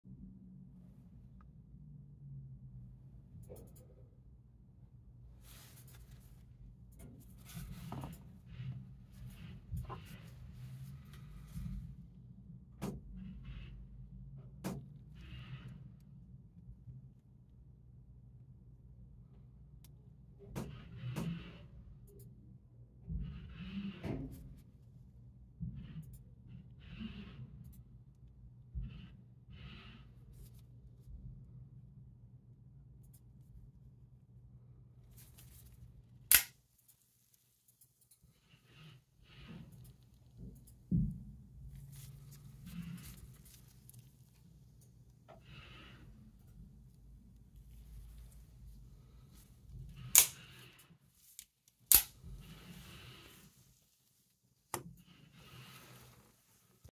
Heretic Colossus tanto stonewash partial serrated
Heretic Knives Colossus Tanto Stonewash Partial Serrated Black Handle

Heretic Knives' Colossus is an Out-the-Front knife with a simple, practical design.
This Heretic knife is strong and lightweight, and it opens and closes easily, making it a great everyday or tactical carry.
The Colossus also features a glass breaker and a deep carry pocket clip.